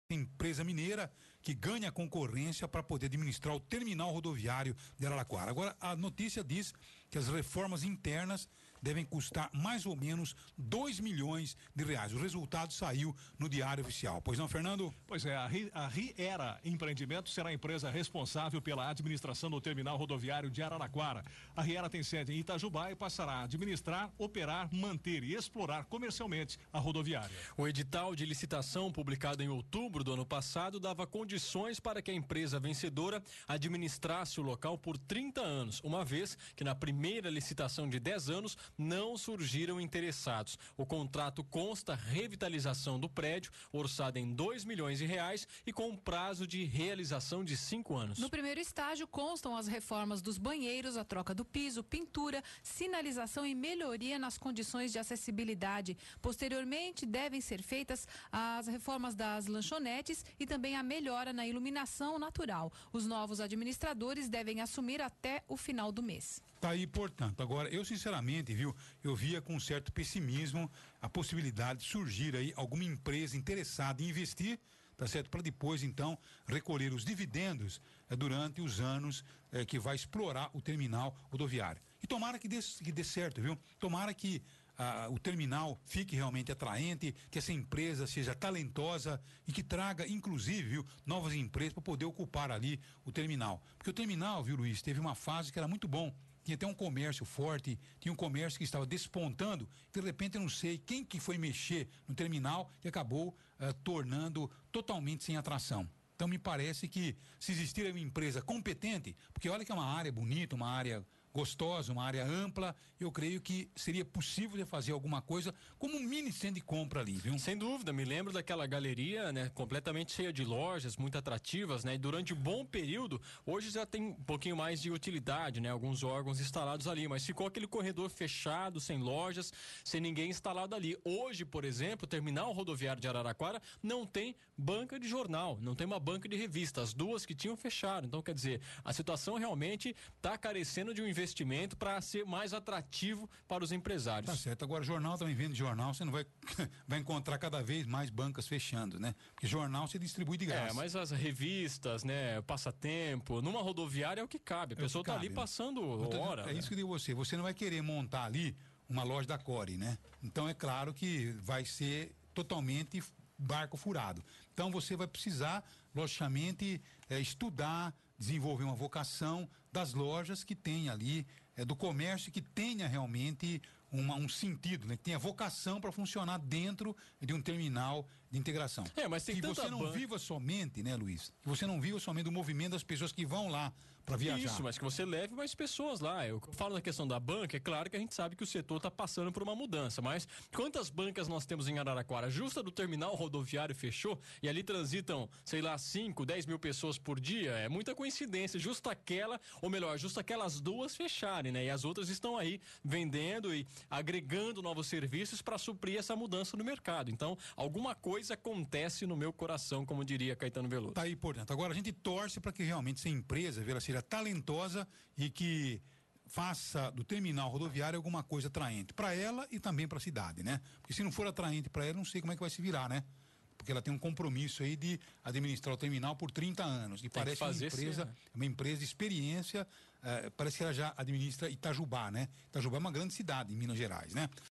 Empresa mineira ganha a concorrência para administrar Terminal Rodoviário
Reformas internas devem custar mais de 2 milhões. O resultado foi publicado na edição do Diário Oficial do final da semana. A empresa mineira deve assumir até o final do mês